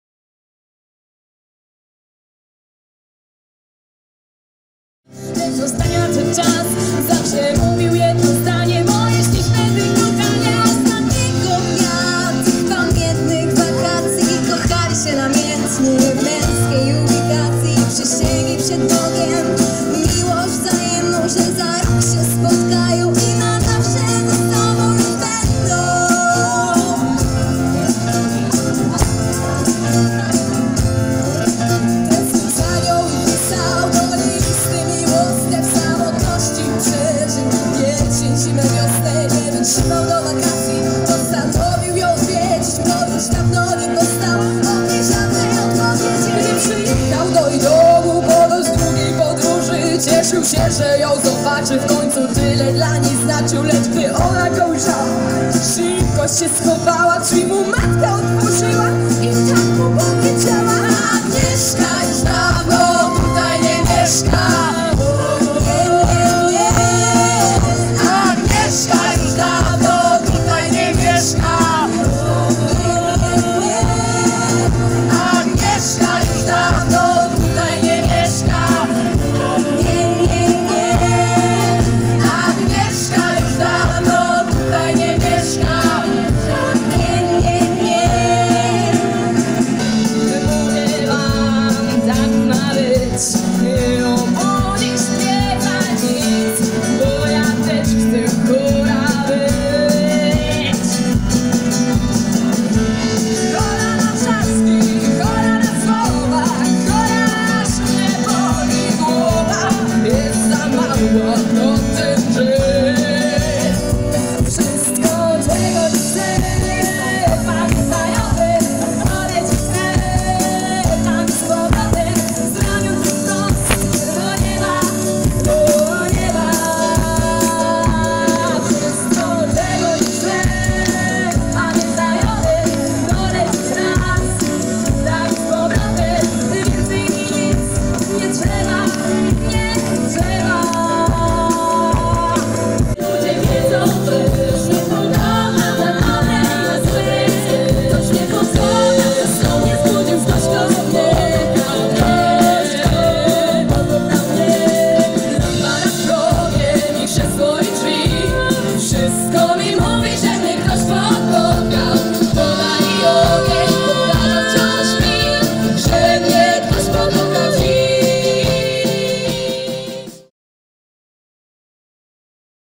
Majówka w Zawieprzycach 2019
W piękny majówkowy dzień zagraliśmy koncert w Zawieprzycach. Zaprezentowaliśmy tam kilka znanych piosenek, część z nich można usłyszeć na filmiku. Na tym koncercie wystąpiliśmy w składzie: Paulina Mazurek(pianino, instruktor), Dorota Jemielniak(wokal), Aleksandra Jóźwiak(wokal), Karolina Wziątek(wokal), Gabriela Sugier(wokal), Paweł Jabłoniec(gitara), Karol Skibiński(perkusja). Zapraszamy na kolejne nasze koncerty :)